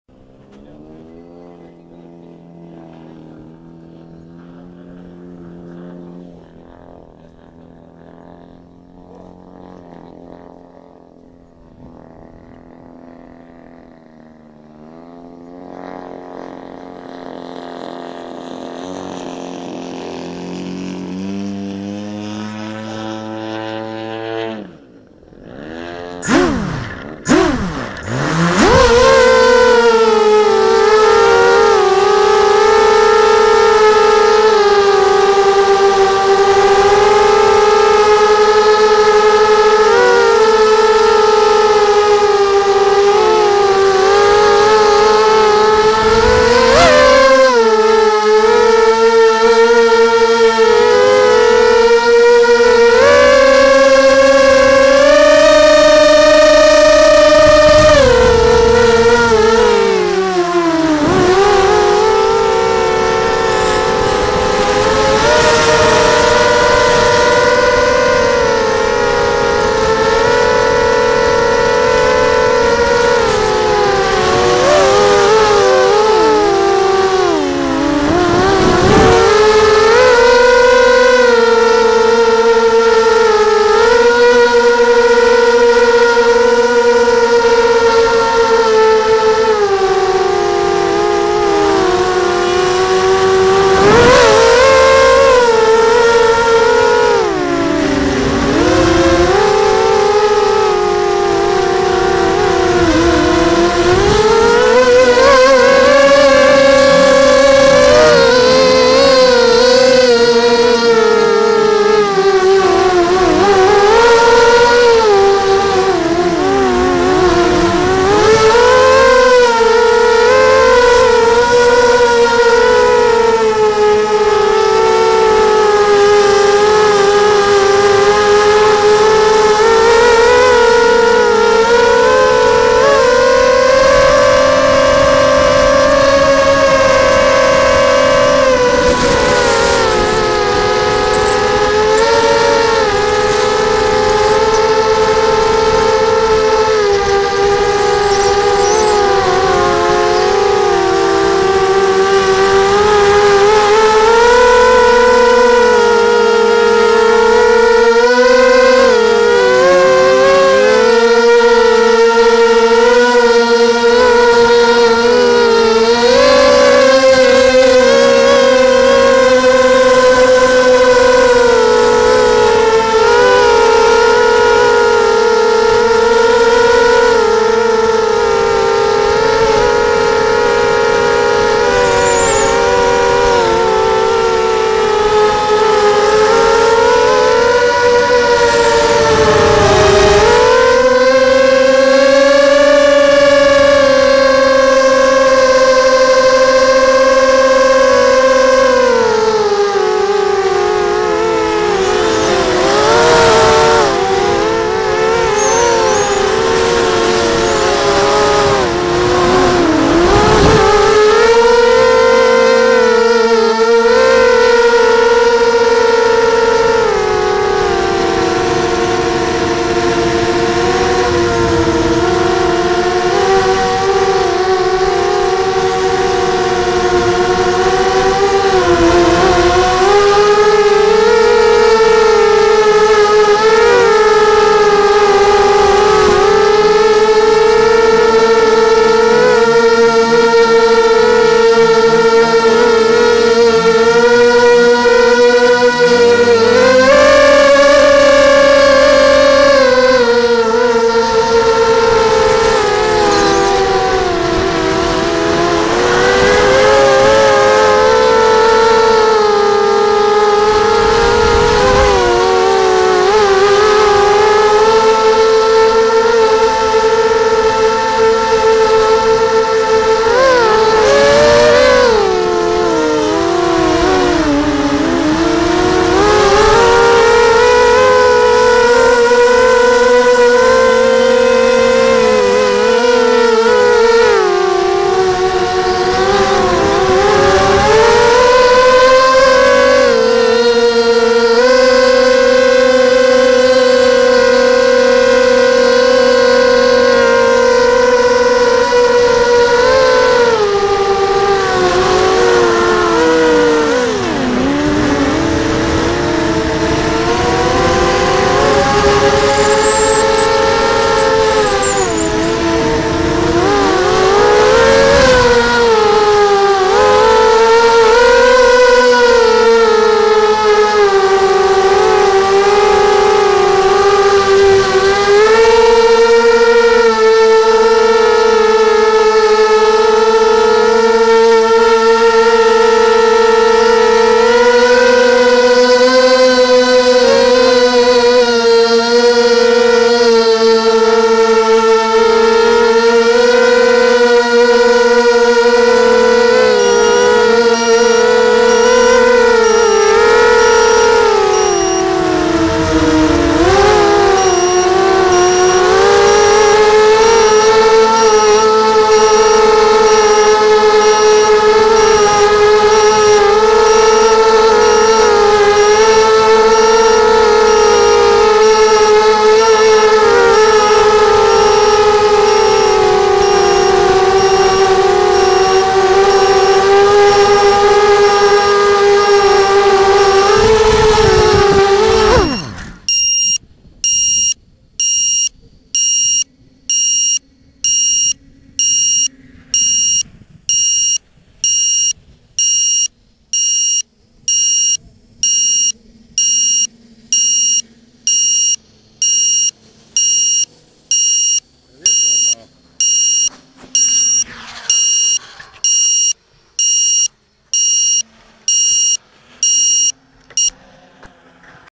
QAV250 FPV Catching Blake Brown With his 100cc Slick
This is me getting a little video with my QAV250 mini H Quad of Blake Brown. First time I've felt comfortable enuff with it to crowd a 4k 100cc airplanes air space...lol..enjoy..